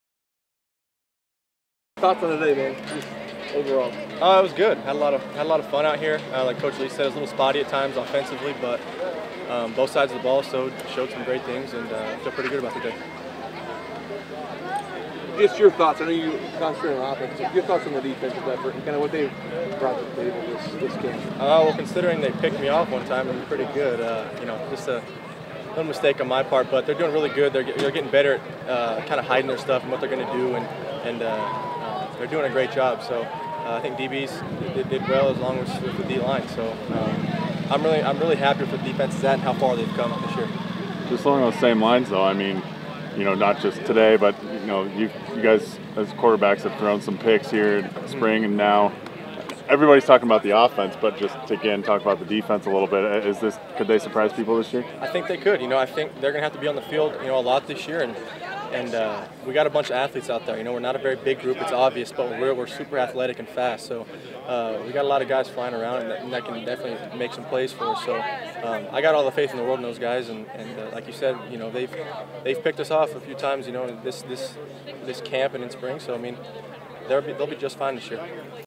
WSU Football: Jeff Tuel after scrimmage. Aug. 11
Hear from Jeff Tuel after today's scrimmage.  GO COUGS!